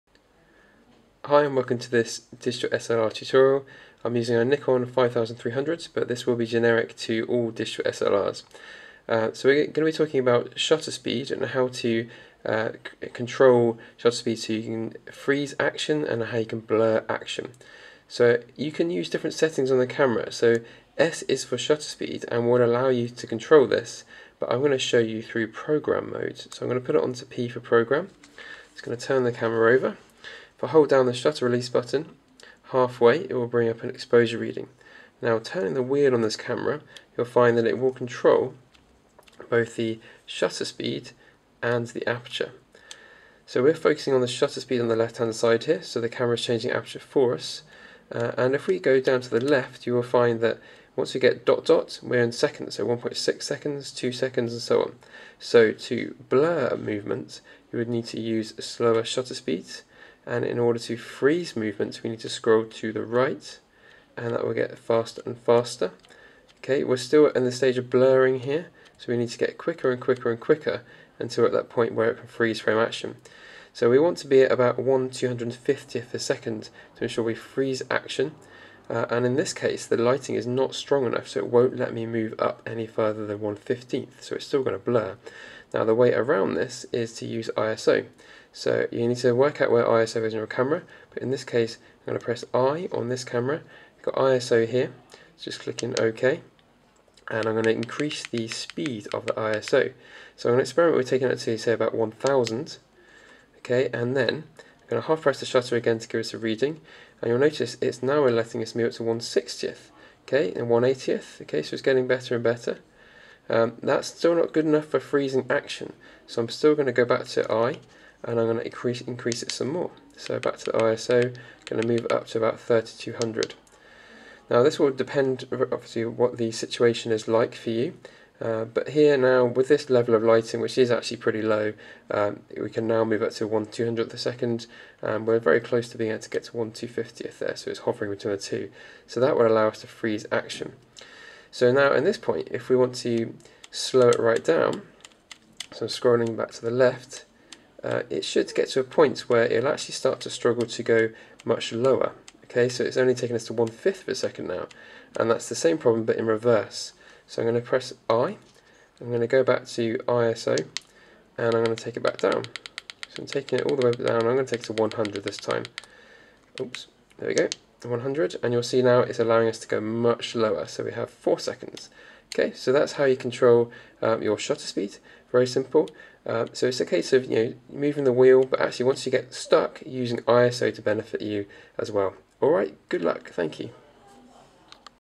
Digital SLR - Shutter Speed (simple tutorial)
This tutorial provides some simple information for changing the shutter speed on your digital SLR. The positioning of the controls will vary from camera to camera but this will give you a good start.